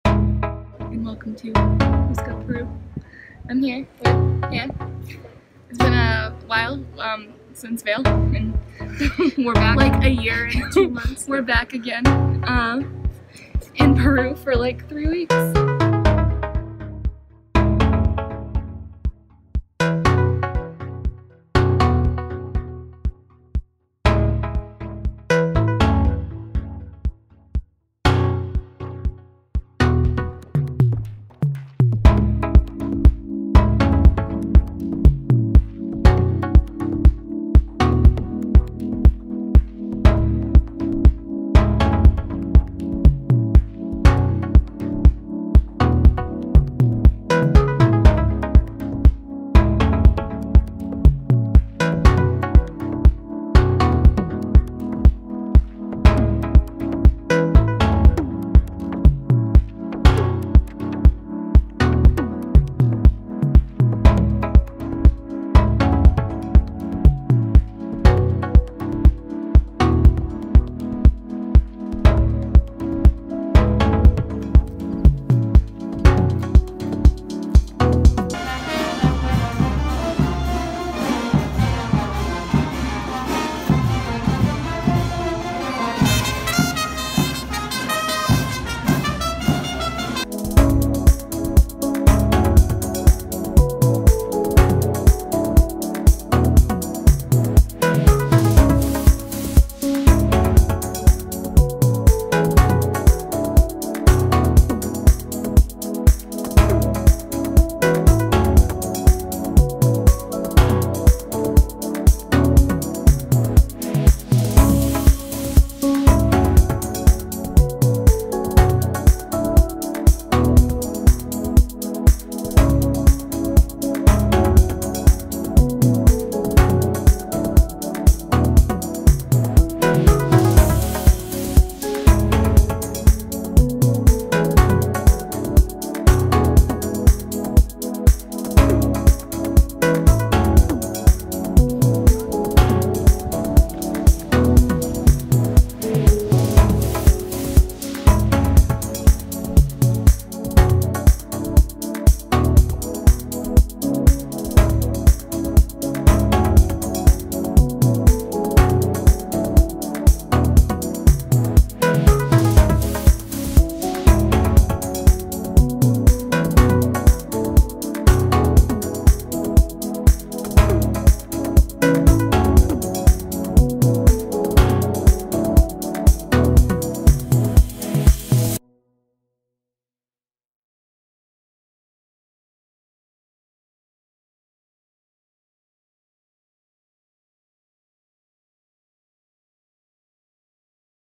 hangin' with the llamas // Peru 2019
Back in July, I headed down to Peru with Han. We started in Cusco and ended in Lima, via Puno, Arequipa, & Paracas. Unfortunately we both forgot how to vlog, so this is what we have.